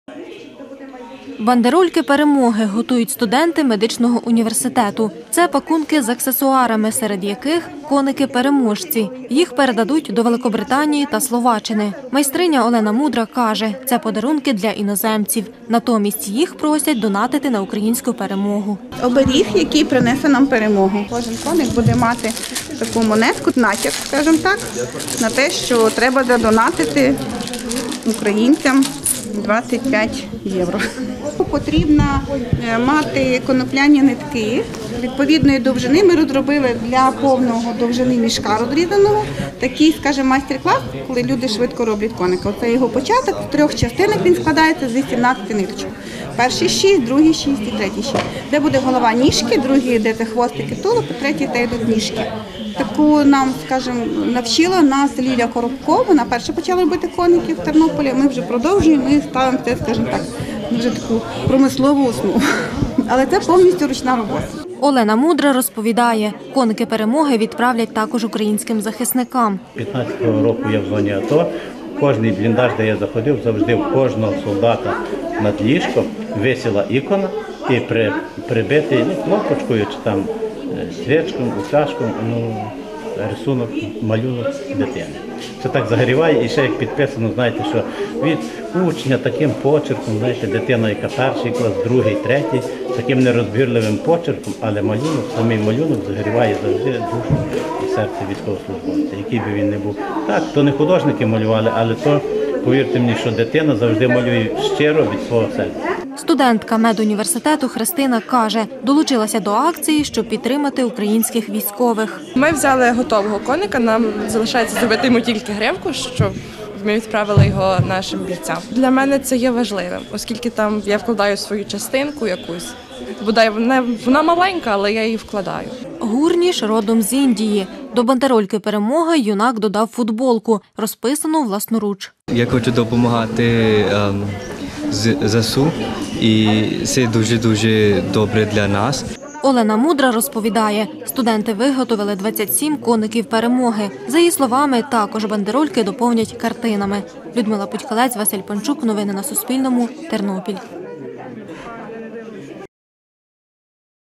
У Тернополі студенти виготовляли вироби, щоб допомогти ЗСУ
Студенти медичного університету готують "Бандерольки перемоги", які відправлять за кордон, а також українським захисникам.